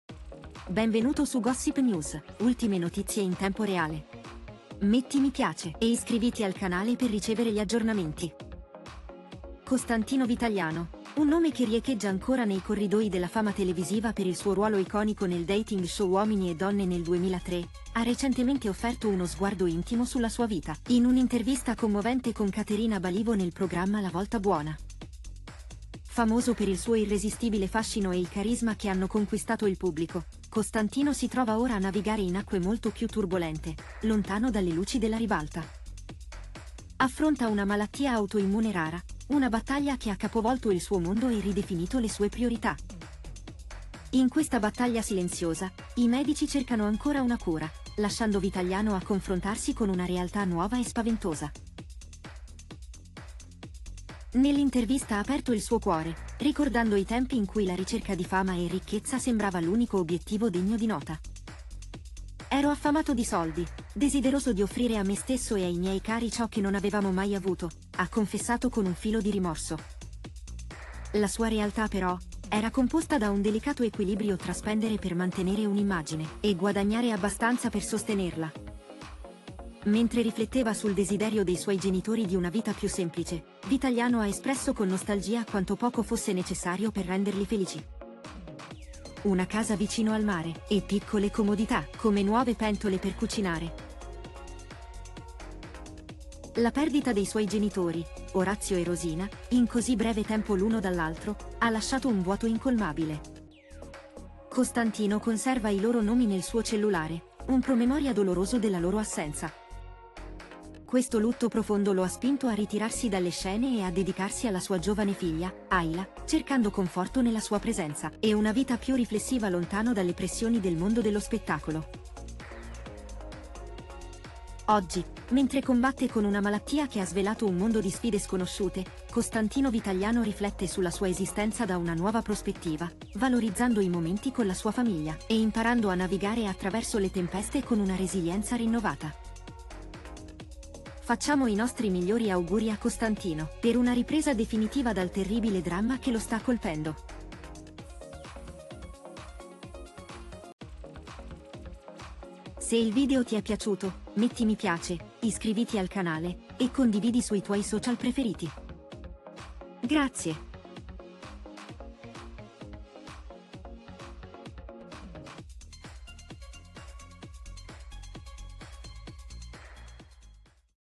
Il Dramma Di Costantino Vitagliano: La Notizia Shock Sulla Malattia!
Costantino Vitagliano racconta il suo dramma, un viaggio toccante tra passato glorioso e le sfide quotidiane contro una malattia rara. Un ritratto di resilienza e amore.